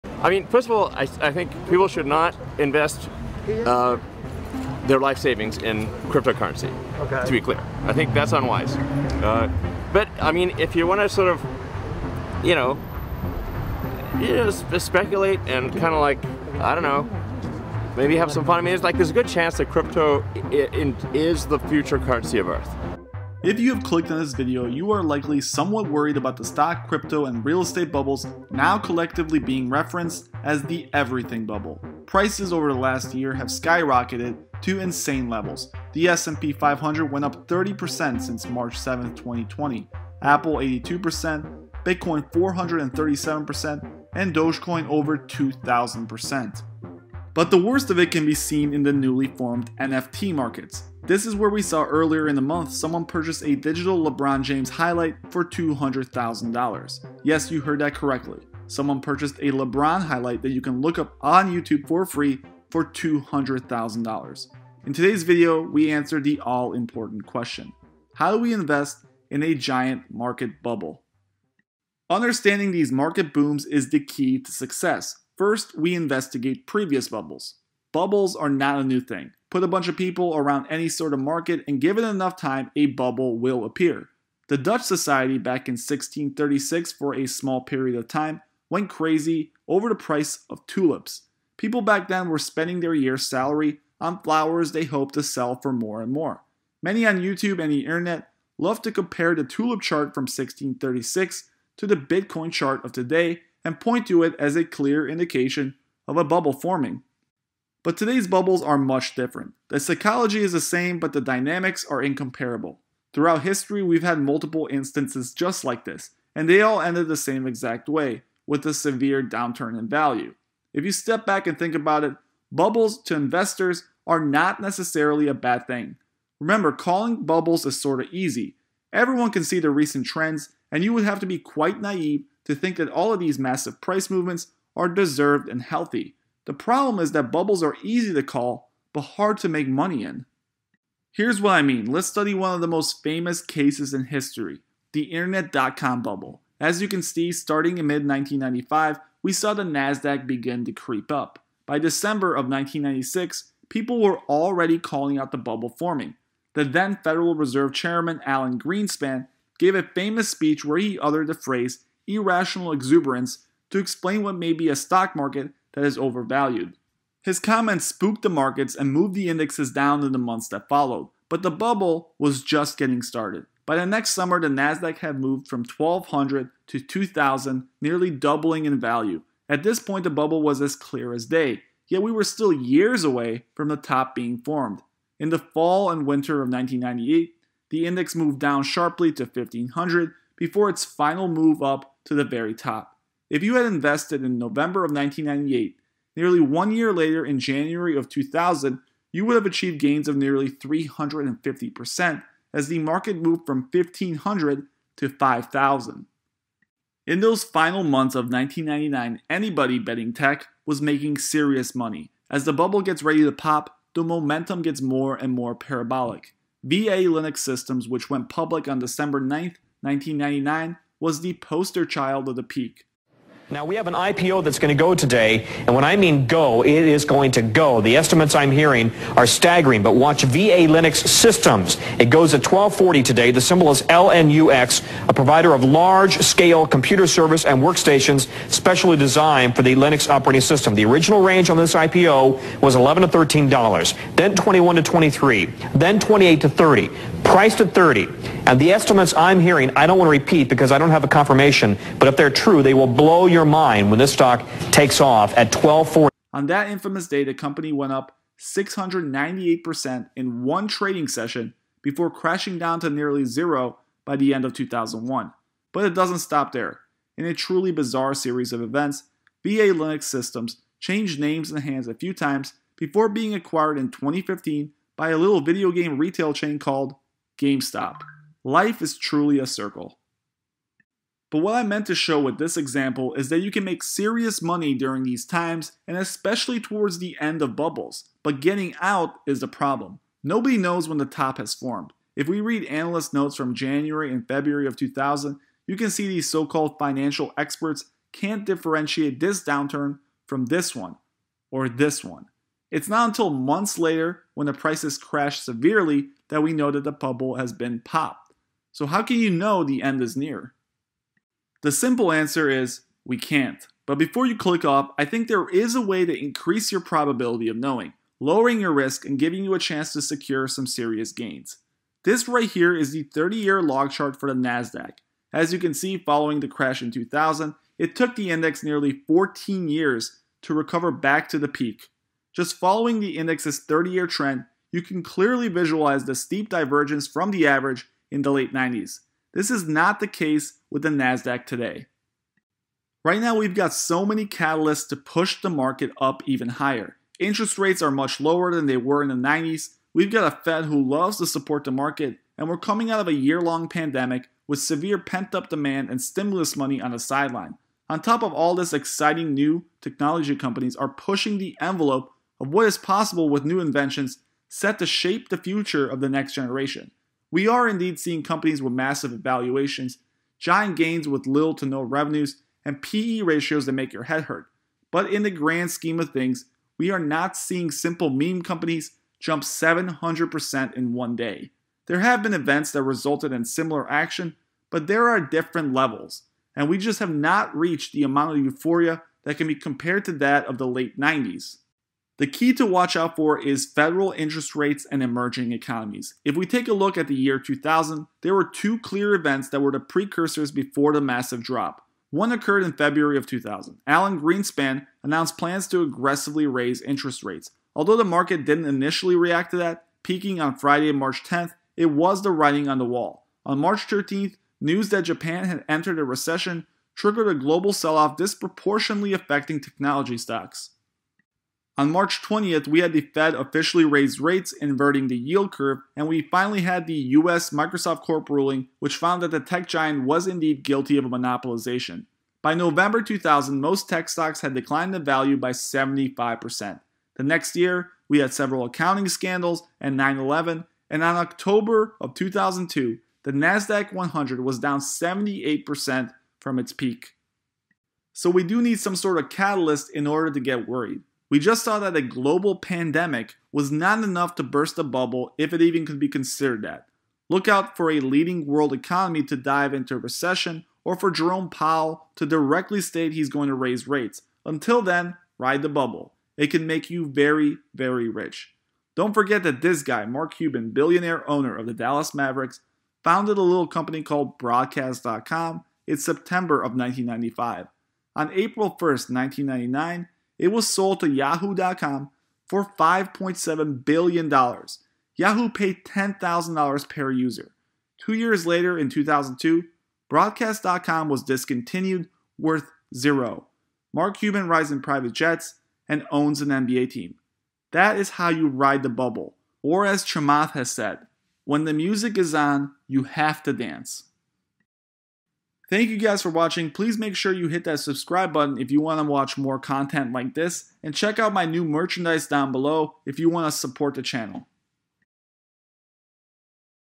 The Everything Bubble
Whether it's Dogecoin, Bitcoin, Gamestop, or Tesla one thing is certain, investors are taking on more and more speculation and risk. In today's video, we discuss whether or not we are in a stock market bubble and look at previous bubbles to investigate how we can profit from them.